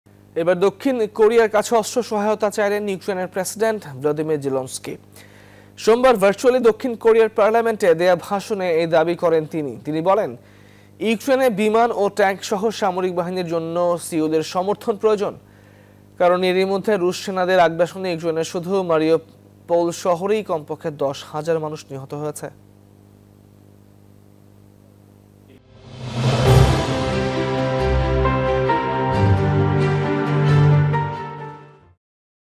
দক্ষিণ কোরিয়ার কাছে অস্ত্র সহায়তা চাইলেন জেলেনস্কি 12Apr.22 | South Korea | Ukraine | Zelensky | War
এবার দক্ষিণ কোরিয়ার কাছে অস্ত্র সহায়তা চাইলেন ইউক্রেনের প্রেসিডেন্ট ভলোদিমির জেলেনস্কি। সোমবার ভার্চুয়ালি দক্ষিণ কোরিয়ার পার্লামেন্টে দেয়া ভাষণে এই দাবি করেন তিনি।

তিনি বলেন, ইউক্রেনে বিমান ও ট্যাঙ্কসহ সামরিক বাহিনীর জন্য সিউলের সমর্থন প্রয়োজন। কারণ এরইমধ্যে রুশ সেনাদের আগ্রাসনে ইউক্রেনের শুধু মারিওপোল শহরেই কমপক্ষে ১০ হাজার মানুষ নিহত হয়েছে।

যুদ্ধ শুরুর পর ইউক্রেনের বিভিন্ন শহরে রাশিয়ার নৃশংসতা  বহু মানুষের প্রাণ গেছে। রুশ সেনাদের সঙ্গে লড়তে আমদের আরো অস্ত্র প্রয়োজন। দক্ষিণ কোরিয়ার অনেক অস্ত্র রয়েছে, যা শুধু সাধারণ ইউক্রেনীয়দের জীবন বাঁচাতেই সাহায্য করবে না, রাশিয়াকে অন্য দেশ থেকে আক্রমণ করা থেকে রক্ষা করতে সহায়তা করবে।

Our other YouTube channels :
News24 Official Address:
NEWS24
371/A
Bashundhara Road
Block – D
Bashundhara Residential Area
Dhaka - 1229